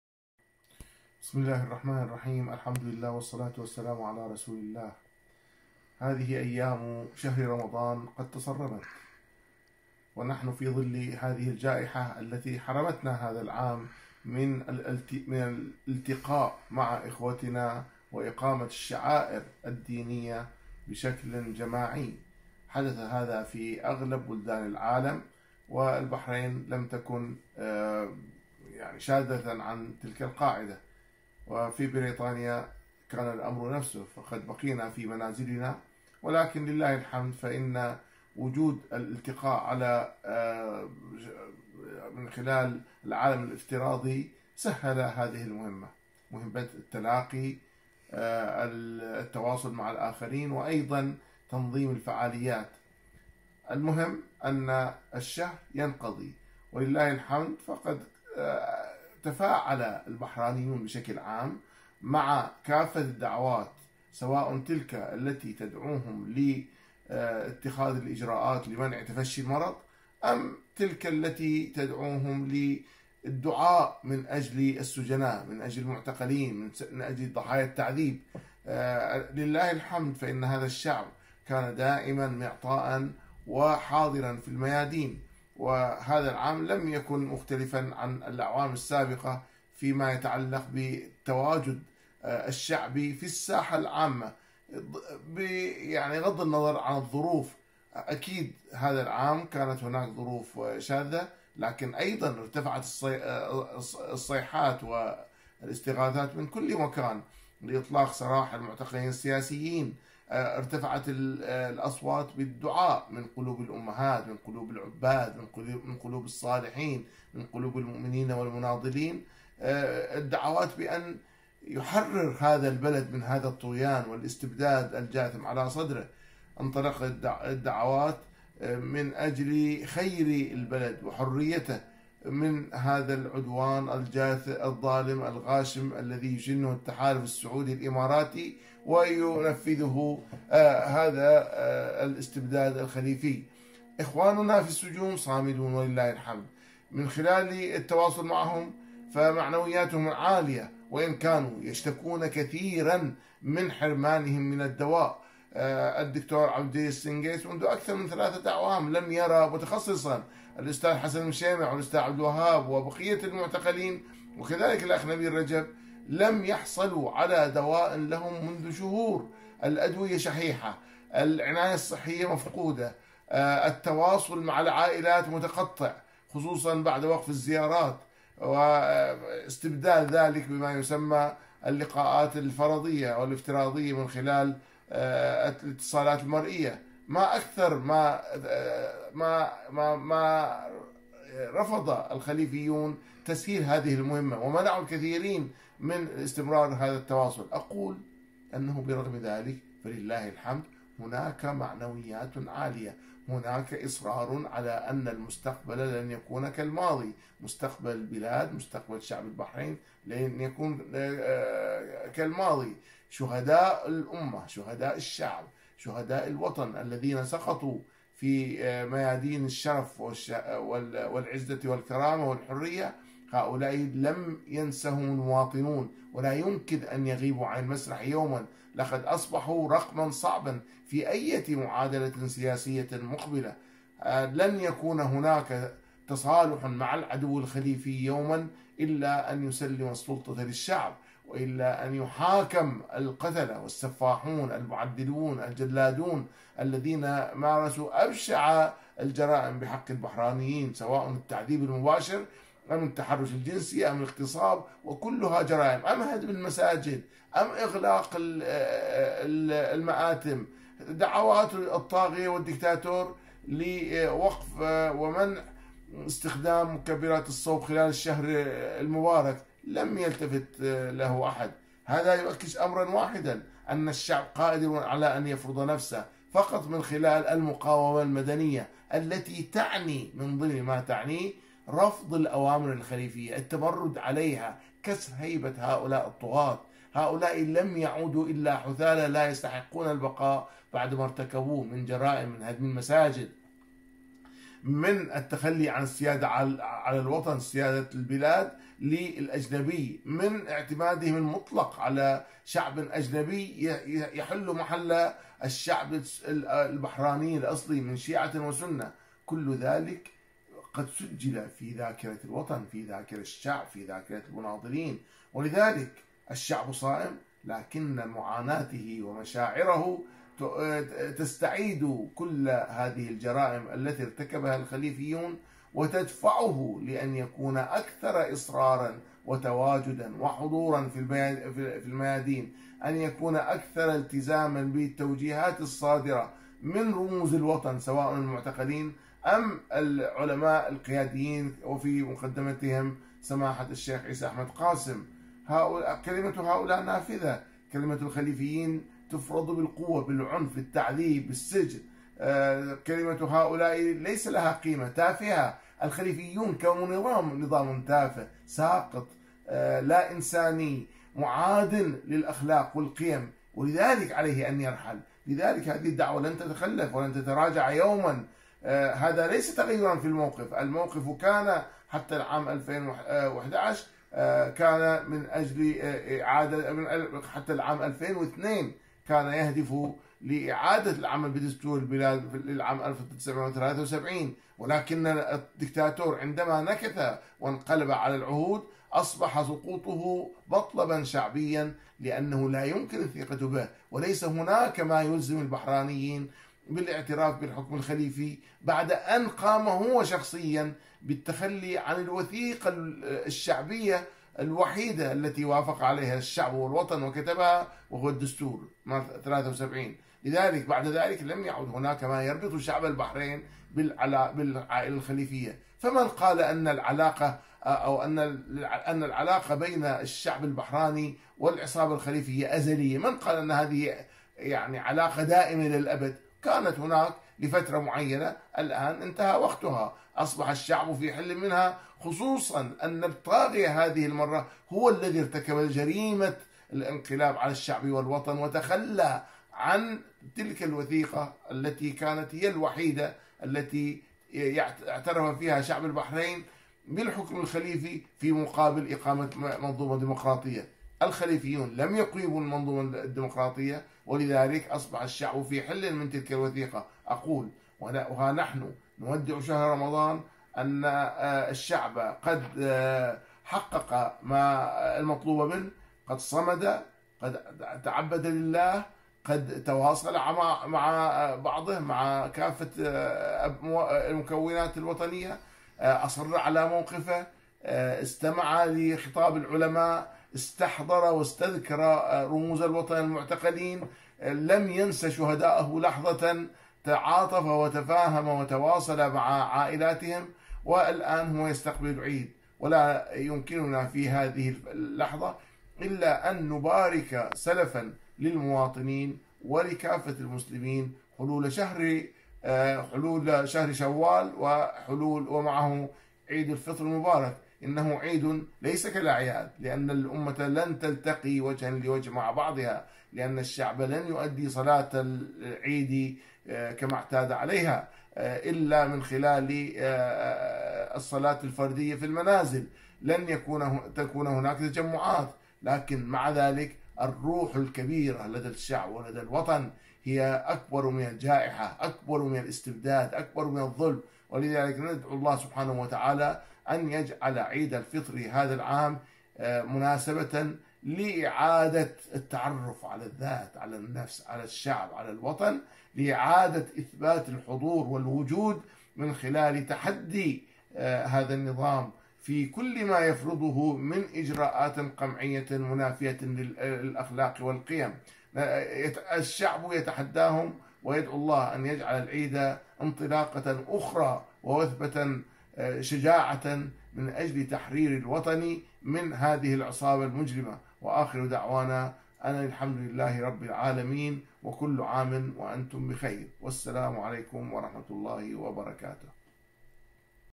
سعيد الشهابي يدعو البحرانيين إلى المقاومة المدنية عبر عصيان أوامر النظام الخليفي والتمرد عليها
وجه المعارض البحراني البارز الدكتور سعيد الشهابي كلمة إلى الشعب البحراني مع قرب حلول عيد الفطر السعيد، بارك فيها هذه المناسبة للشعب وللأمة الإسلامية، محييا صمود الشعب البحراني وثباته في المواجهة مع النظام الخليفي الحاكم في البحرين.

ودعا الشهابي شعب البحرين إلى المقاومة المدنية عبر عصيان أوامر النظام الخليفي والتمرد عليها، واصفا الخليفيين بالحثالة.

أشاد الشهابي بالشعب الذي لايزال حاضرا في الميادين حيث الحضور الجماهيري في ساحات المواجهة مع النظام داخليا وخارجيا، وارتفاع الأصوات المطالبة بإطلاق سراح السجناء وبوقف العدوان السعودي الإماراتي على اليمن.

حيا الشهابي المعتقلين الصامدين في سجون البحرين مشيدا بمعنوياتهم العالية بالرغم من ظروف السجن السيئة حيث الحرمان من العلاج اللازم والعناية الصحية المناسبة والحرمان من الزيارات العائلية.

أكد المعارض البحراني البارز على ان المستقبل لن يكون كالماضي، لافتا إلى أن ”الشهداء الذين سقطوا لن ينساهم المواطنون وأصبحوا رقما صعبا“ مشددا على ألا تصالح مع الخليفيين مشترطا ان يسلموا الحكم للشعب وأن يحاكم من وصفهم بالسفاحين والجلادين والمعذبين الذين مارسوا أبشع الجرائم بحق البحرانيين.

طالب الشهابي الشعب بأن ينتهج خيار “المقاومة المدنية من خلال رفض الأوامر الخليفية والتمرد عليها و كسر هيبة هؤلاء الطغاة الذين لم يعودوا سوى حثالة بعد مارتكبوه من جرائم“.

أشار الشهابي إلى هدم الخليفيين للمساجد وتخليهم عن سيادة البلاد والاعتماد على الأجانب واستبدال الشعب بالأغراب مؤكدا على ان هذه الجرائم ”مسجلة في ذاكرة الوطن“.

وأضاف ”فالشعب يستذكر هذه الجرائم التي تدفعه ليكون أكثر إصرارا على مطالبه، والتزاما بتوجيهات قادته ومنهم آية الله قاسم، فكلمتهم نافذة وأما الخليفيون فيفرضون كلمتهم بالقوة“.

وصف الشهابي النظام الحاكم في البحرين بالسافل مطالبا إياه بالرحيل، لافتا إلى أن النظام الخليفي انقلب على شعب البحرين بعد أن تخلي عن الوثيقة الشعبية التي أقرها الشعب وهي دستور ١٩٧٣.

وأكد الشهابي على أن العلاقة بين الشعب والخليفيين أزلية بل إن الشعب أصبح في حل منها بعد ”انقلاب الطاغية على الدستور الذي اقر فيه الشعب تلك العلاقة مقابل الديمقراطية لكن الشعب اصبح في حل منها بعد انقلاب الخليفيين“.

واختتم المعارض البحراني البارز كلمته بالقول إن ”العيد مناسبة لإثبات الوجود من خلال تحدي النظام الخليفي من أجل تحرير الوطن من العصابة المجرمة.“